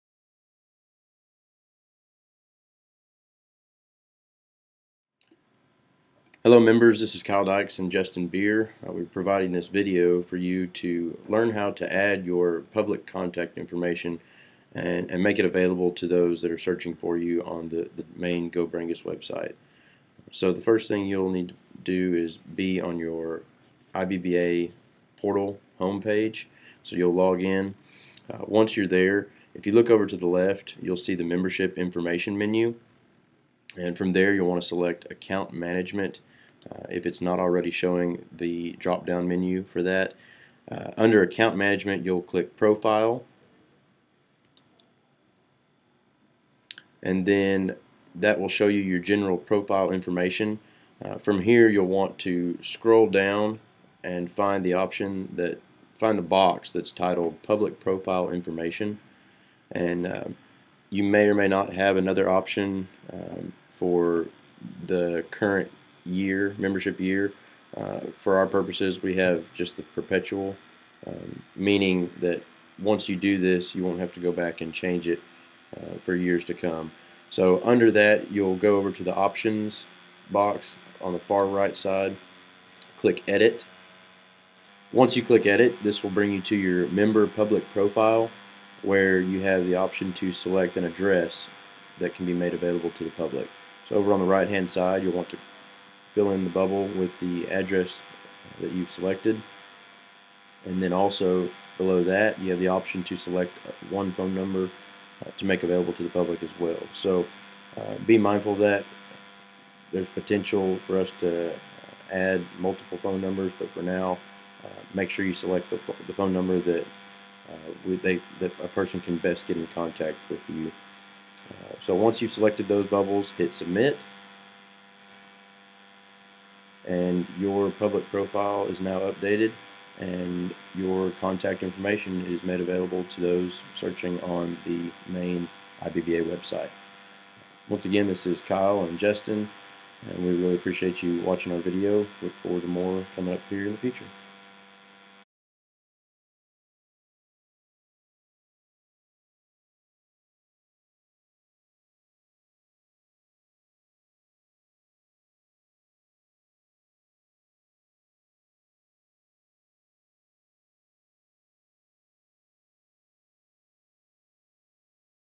Public Profile Intructional Video
An outline of the steps is also provided below:

Step 1:
Go to the main portal page and log in to your account.

Step 2:
Click on Profile, this will be the first option you see within Account Management.

Step 3:
This will bring you to a page with a lot of information, Scroll Down until you find a box with the title “Public Profile Information”. Over on the right, you will see the Options box, Click Edit.

Step 4:
This will bring you to your Member Public Profile page, Find the Address You Want to Make Public, Click the Circle Button on the right next to the Address.

Step 5:
Double Check the right address has been selected, now Click Submit.